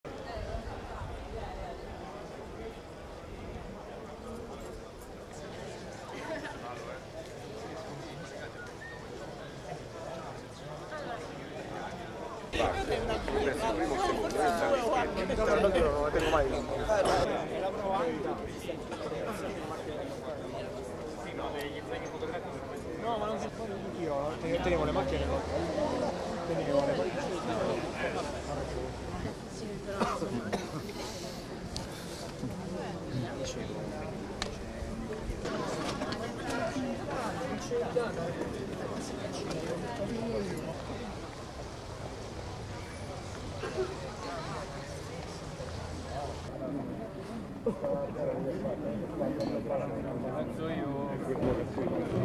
MARINO RITIRA LE DIMISSIONI: LA DESTRA ROMANA FUORI DAL CAMPIDOGLIO
(Agenzia VISTA) - Roma, 29 Ottobre 2015 - Ignazio Marino ha ritirato le dimissioni. La giunta comunale è riunita per decidere il da farsi.